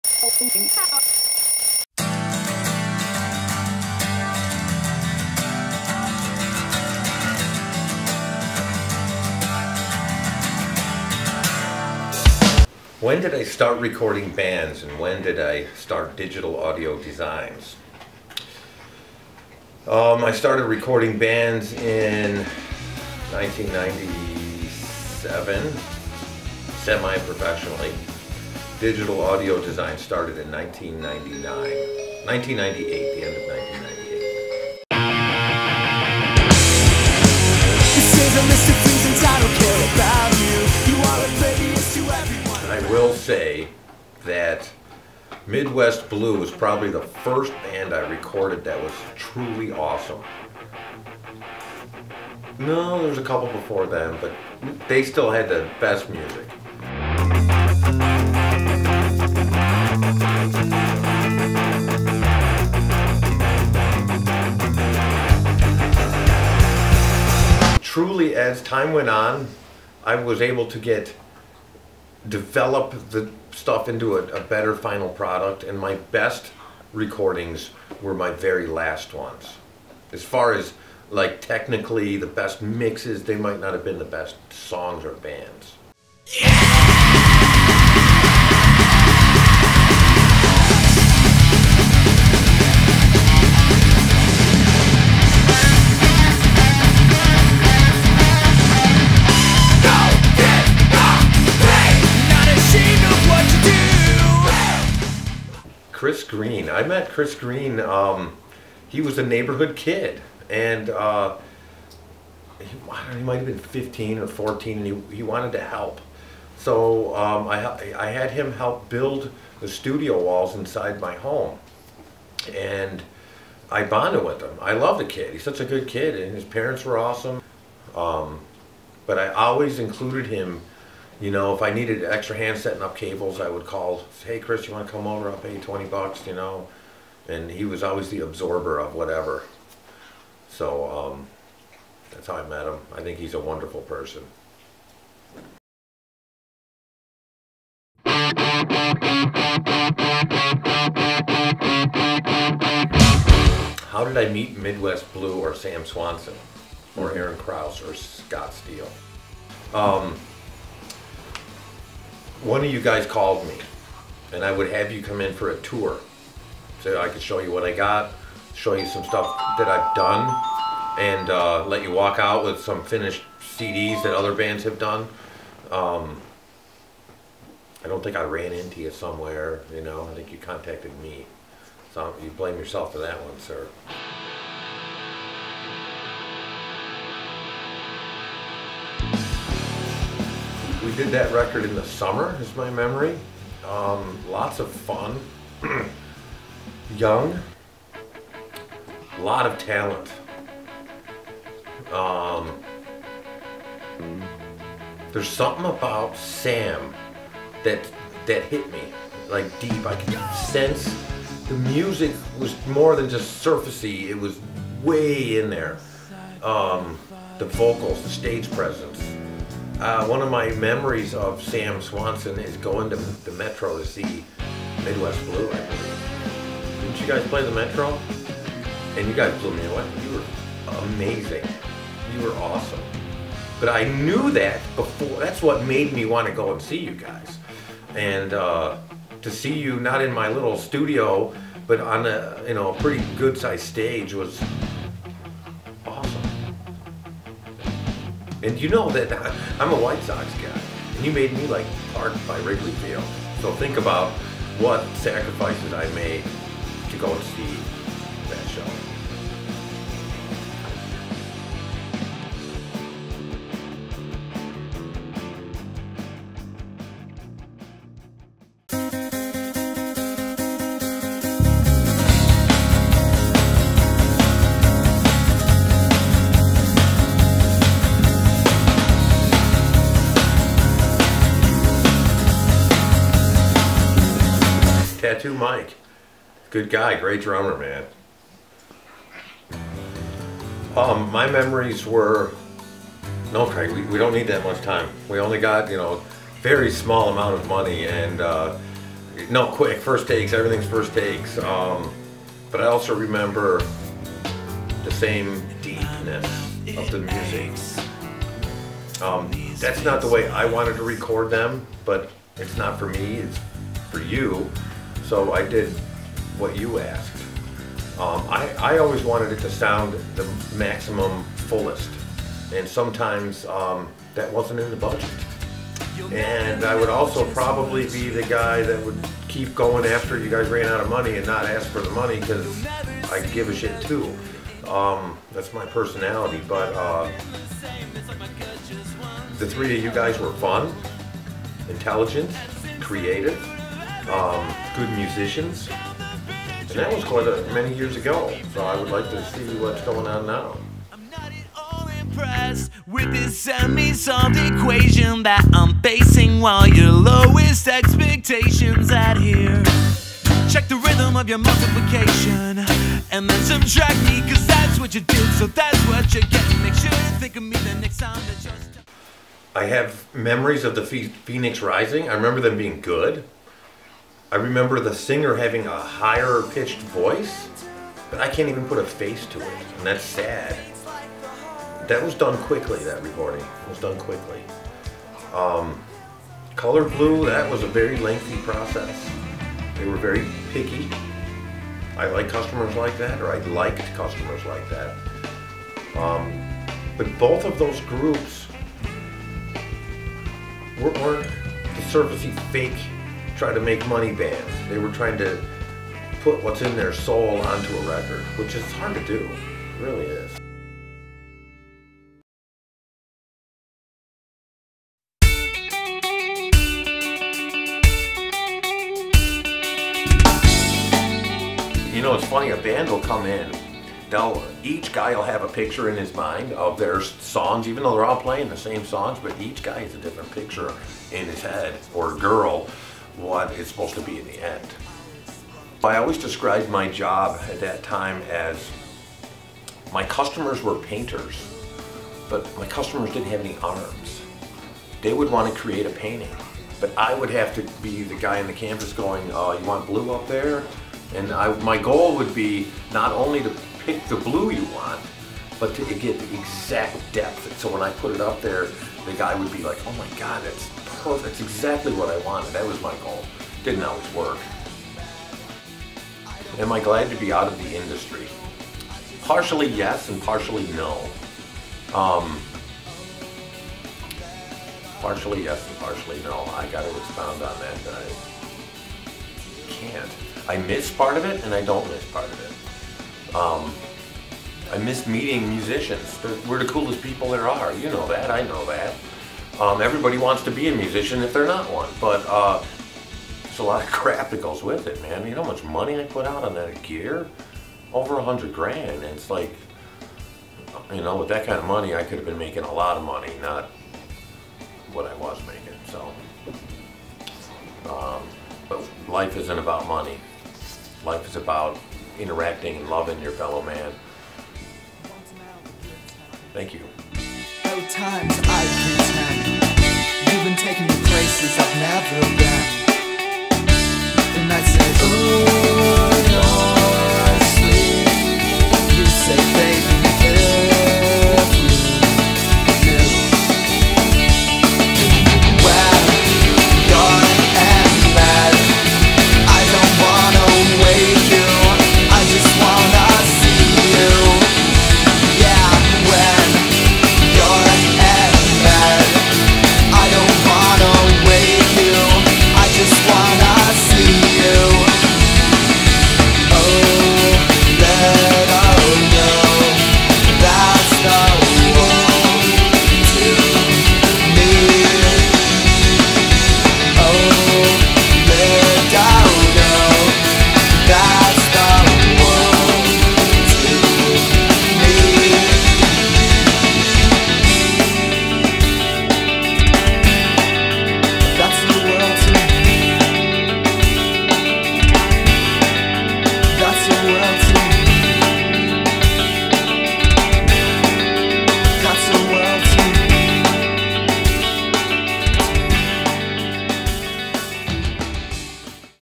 AN INTERVIEW WITH CRAIG BAKER - Digital Audio Designs - (1998-2008)
This is a short interview with Producer/Engineer/Owner of Digital Audio Designs, Craig Baker.  He has recorded many artists including:  MIDWEST BLUE, DORMLIFE, THE PHOENIX RISING, THE COLOUR BLUE, TOOK TO THE SEA, THREEFOLD, SECOND PATH, THE MILKMAN'S KID and many many others...  Excerpts from this footage will be appearing in ACR's first Full-Length Documentary Film:  "KEEP IT ON THE dL:  the dormlife story" which will take a very long time to complete.  However, this should serve as a nice little teaser of things to come.  This video takes a brief tour of the history of Digital Audio Designs in its decade of existence from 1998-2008 and some of the music recorded and mixed along the way by Craig Baker.  Enjoy!!!